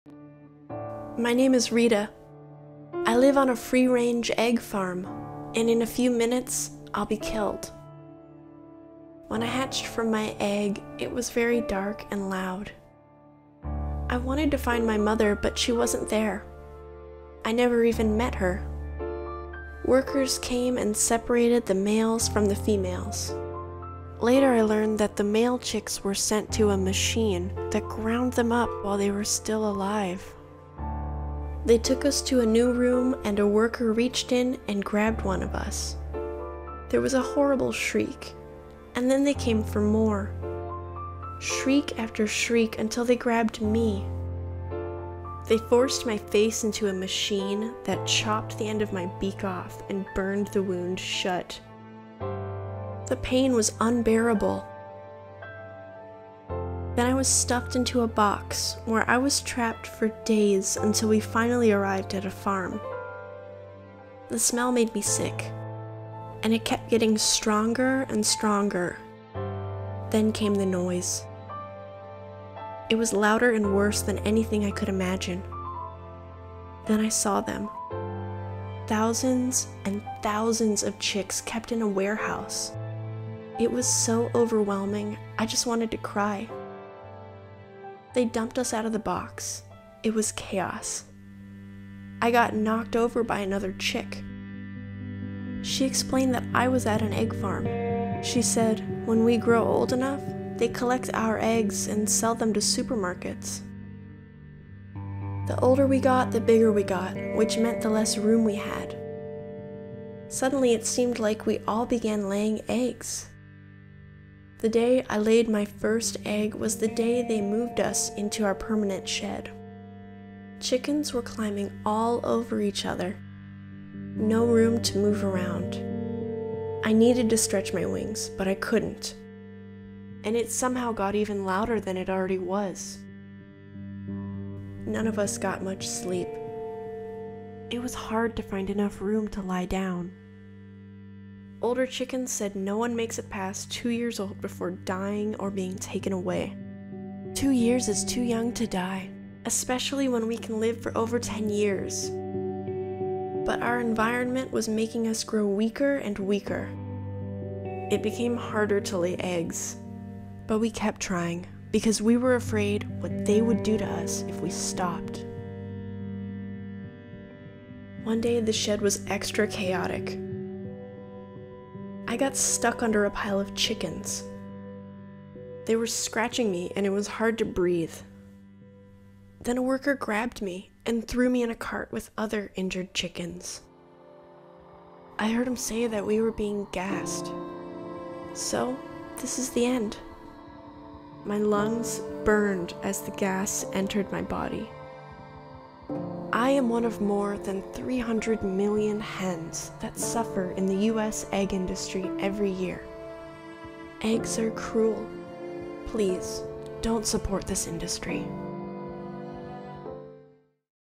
Draw My Life: Hen’s Life in the Egg Industry
Misery, filth, and darkness—this is a hen’s life in the egg industry.  When you see “free-range” eggs, remember Rita gasping for breath in a gas chamber 💔